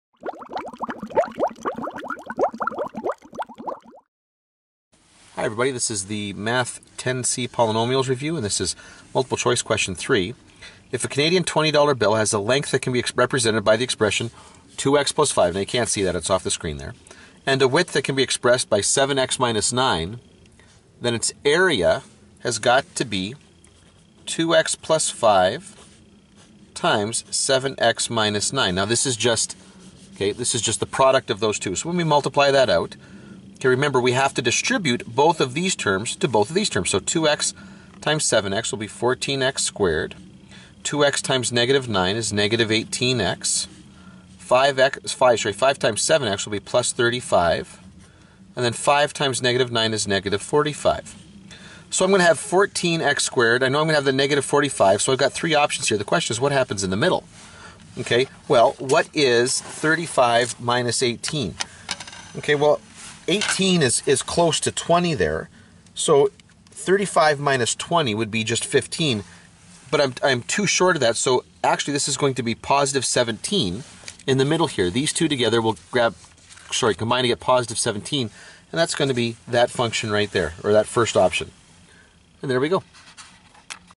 Math 10C Poly Review MC#3
This video is about multiplying binomials.